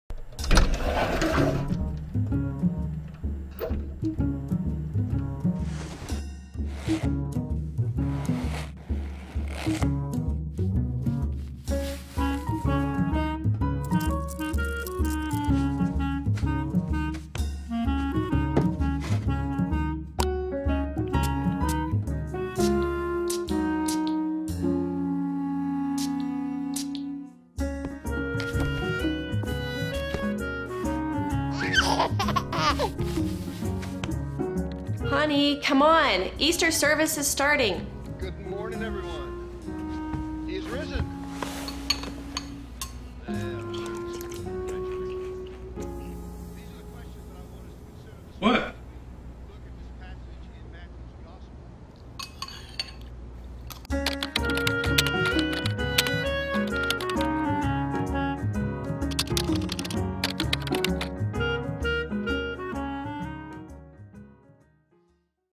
Easter Advertisment
Just because everyone is stuck at home, it doesn't mean you can't still celebrate and join in on celebrating the Resurrection of our Lord and Savior. Please, come join us this Sunday!